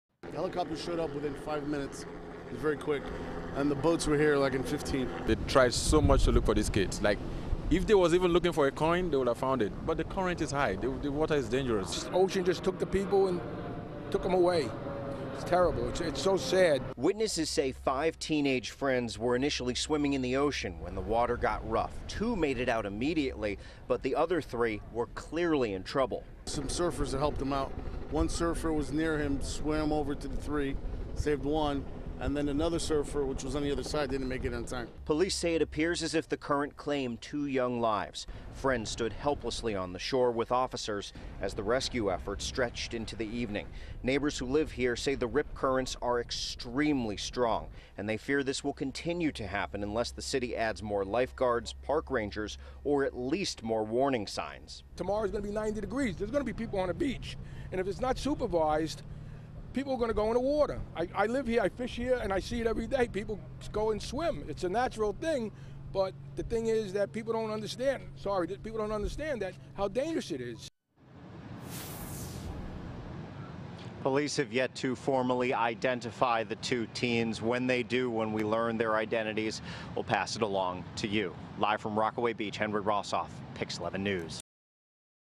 Search underway for missing swimmers at Rockaway Beach
One person has been rescued from the ocean near Beach 96th Street in Rockaway Beach, with two more swimmers still missing, according to FDNY officials.